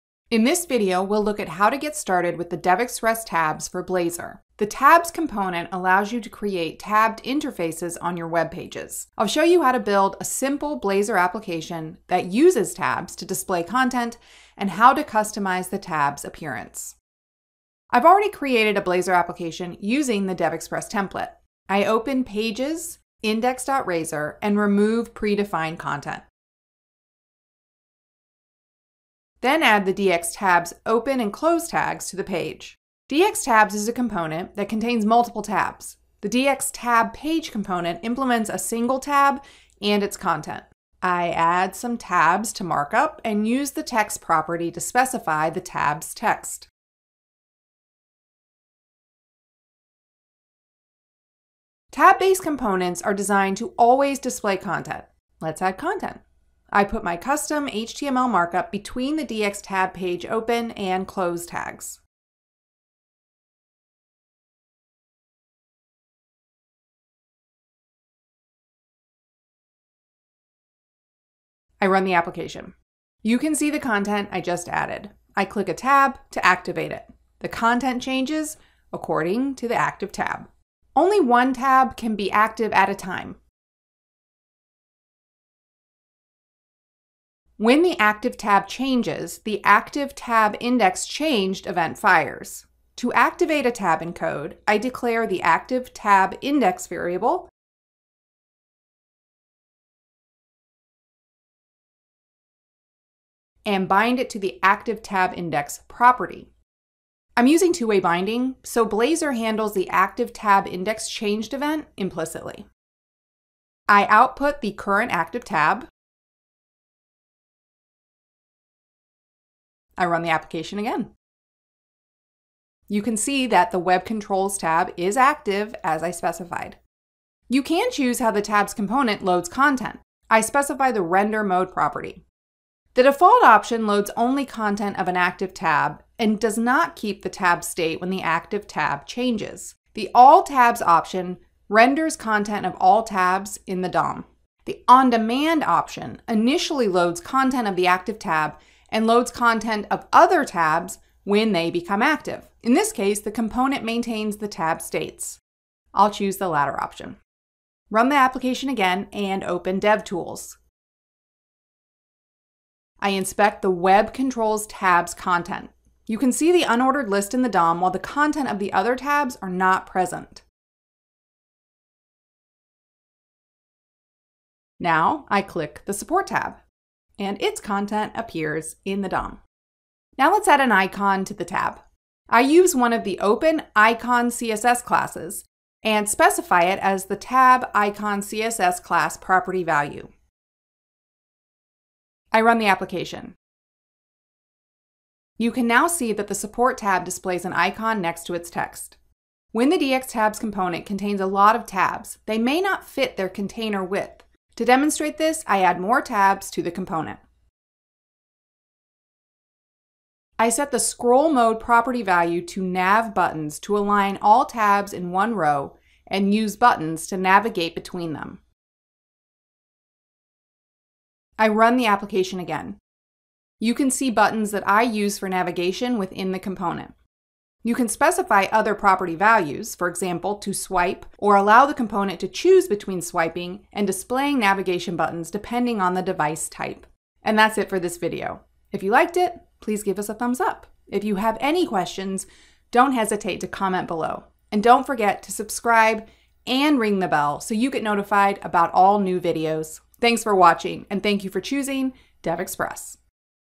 Blazor Tabs: Get Started
This video shows you how to get started with the DevExpress Tabs for Blazor (DxTabs). The DxTabs component allows you to display and manage data in a tabbed format. This tutorial describes how to build a simple Blazor application and display content in tabs.

⏱ Timeline ⏱
0:00 Add the DxTabs component and populate it with tabs
0:52 Add content to tabs
1:24 Activate a tab
2:06 Specify how the component loads its content
3:00 Add a tab’s icon
3:23 Tab adaptivity